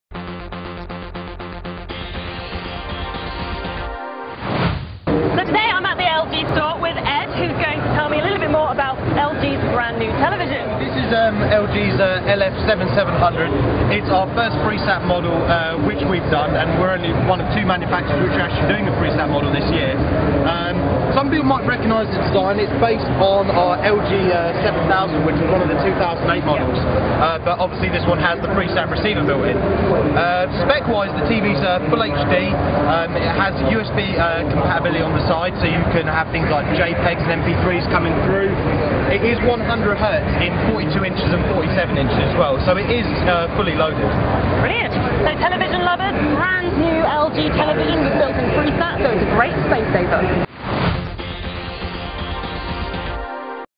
LG: chat with Edd at the Gadget Show
Edd from LG UK being interviewed at the 2009 Gadget Show